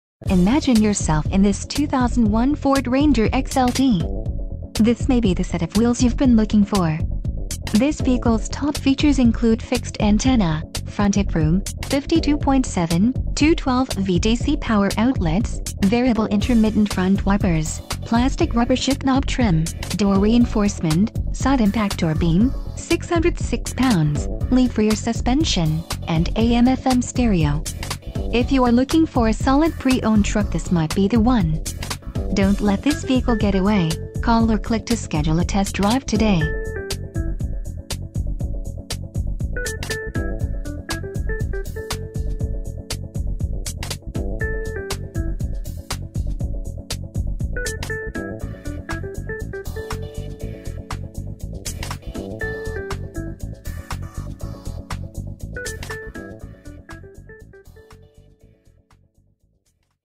2001 FORD RANGER - Tom Denchel's FordCountry.com Prosser -
For Sale in: Prosser, WA

Imagine yourself in this 2001 FORD RANGER XLT. This may be the set of wheels you've been looking for. This vehicles top features include Fixed antenna, Front Hip Room: 52.7, Two 12V DC power outlets, Variable intermittent front wipers, Plastic/rubber shift knob trim, Door reinforcement: Side-impact door beam, 606 lbs., Leaf rear suspension, and AM/FM stereo. If you are looking for a solid pre-owned truck this might be the one. .